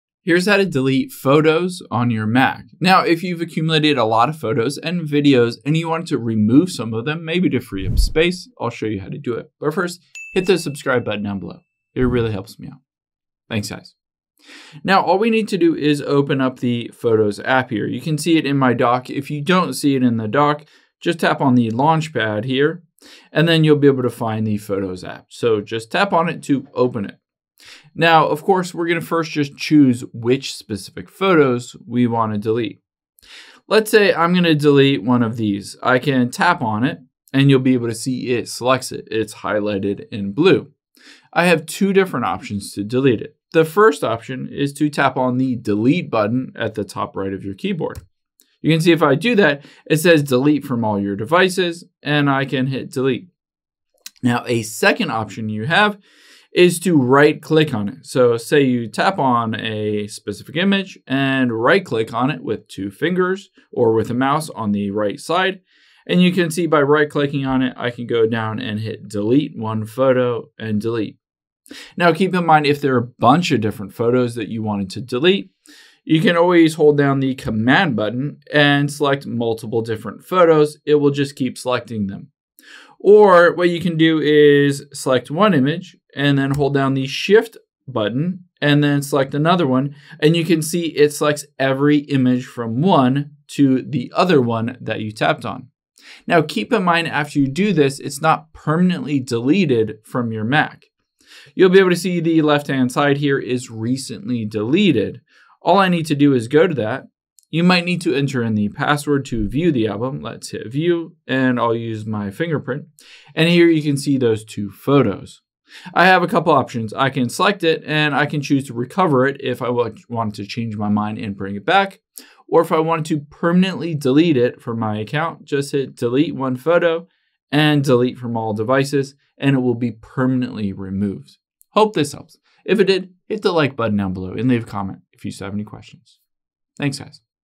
How To Delete Photos On Mac
Here's how you can permanently delete any photos on your MacBook both in the photos app as well as in a folder.